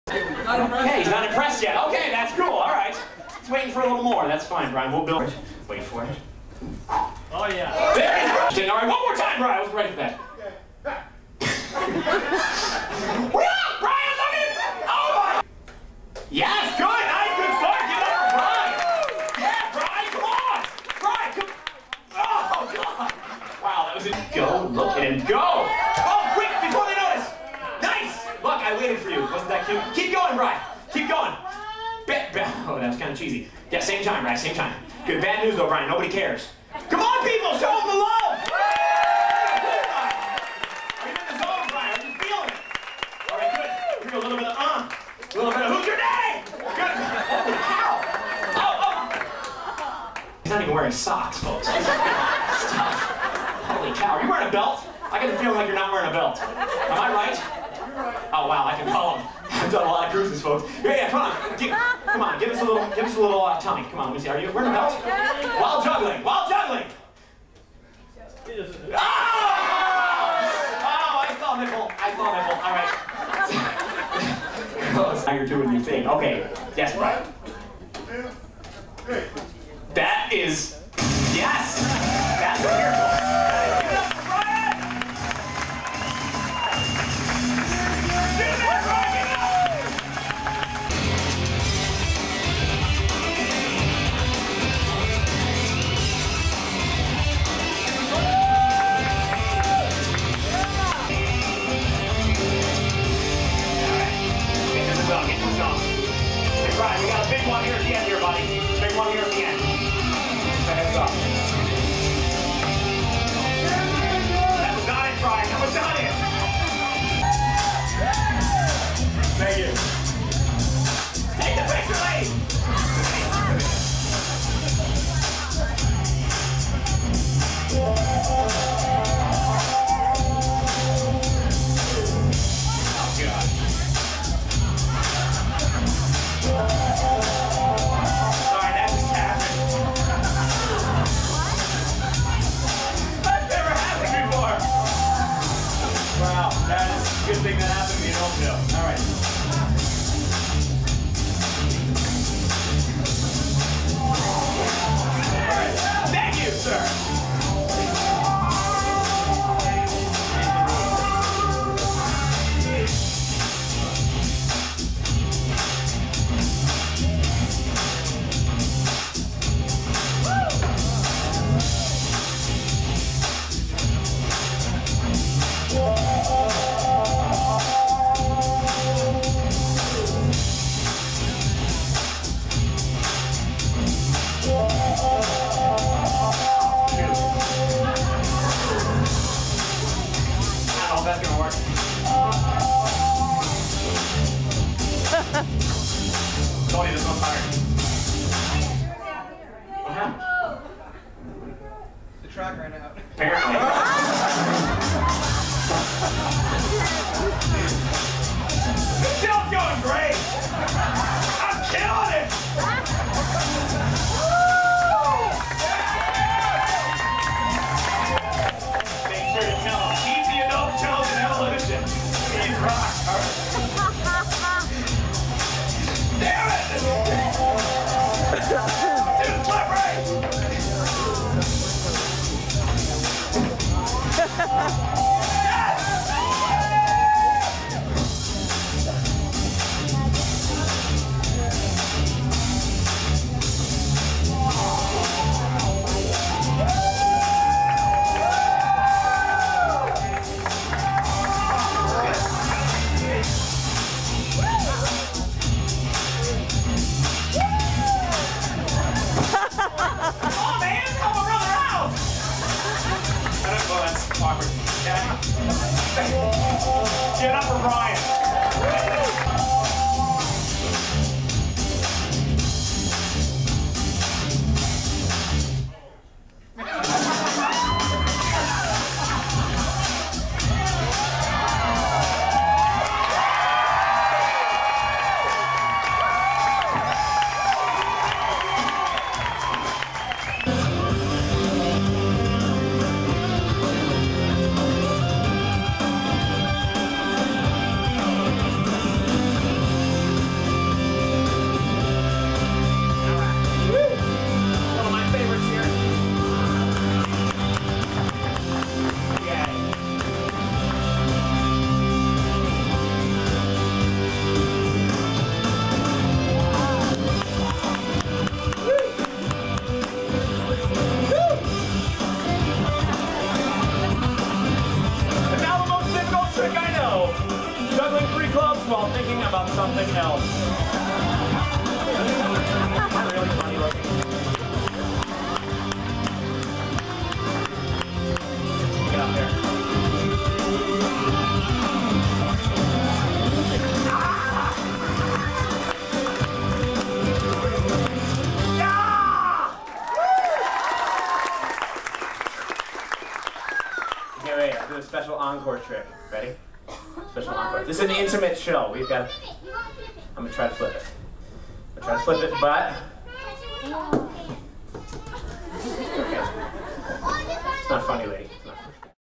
Dream Jeff Civillico
The enormously talented Jeff Civillico! You really have to see this guy in person, he is VERY entertaining! Even though I have seen his show several times, I still laugh my butt off :)